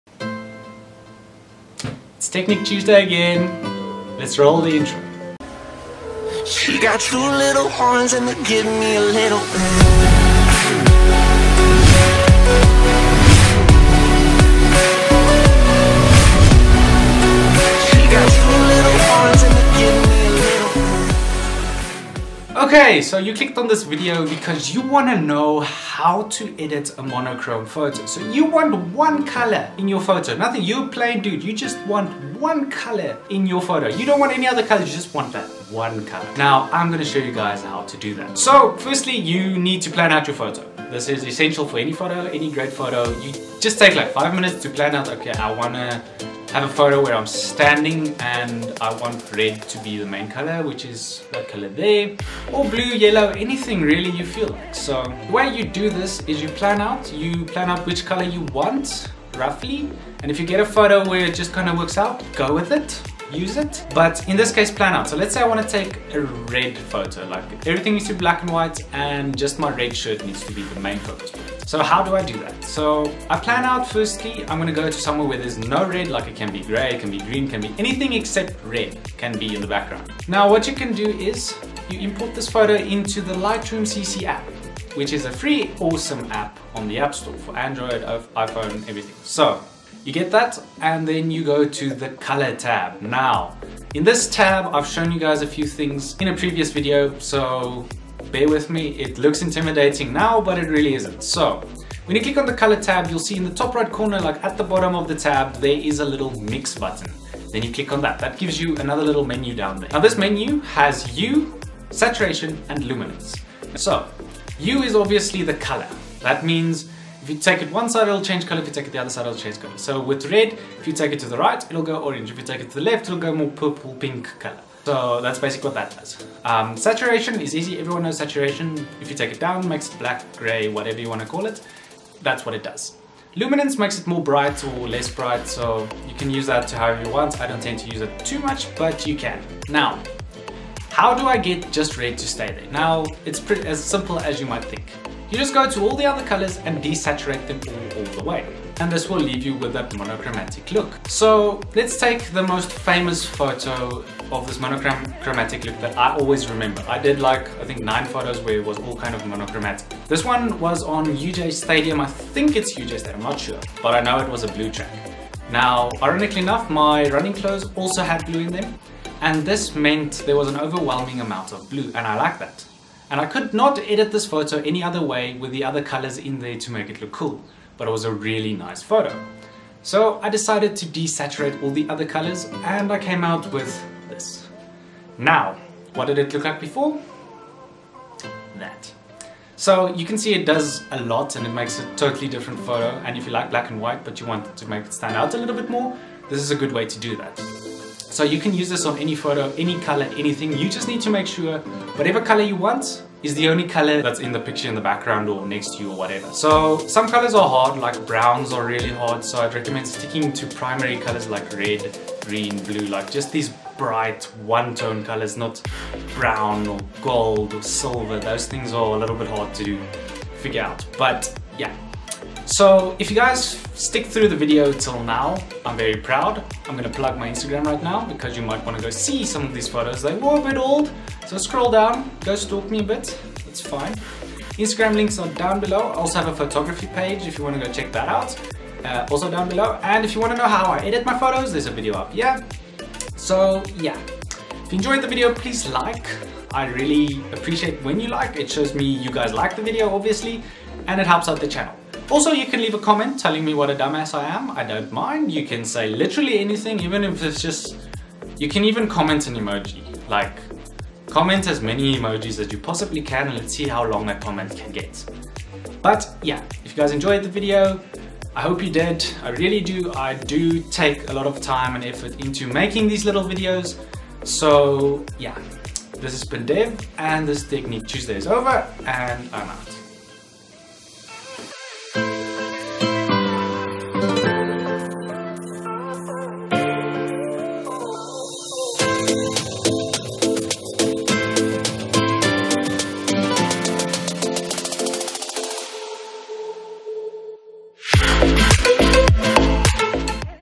HOW TO get that MONOCHROMATIC look | Technique Tuesday
Hey guys it's DeV and in this Technique Tuesday I teach you guys how to get that monochromatic look in lightroom cc (mobile). In this photo editing tutorial I teach you exactly how to get only one saturated color in your pics for that epic instagram look!

MY GEAR:
Canon 200D
Gorilla pod
Iphone 8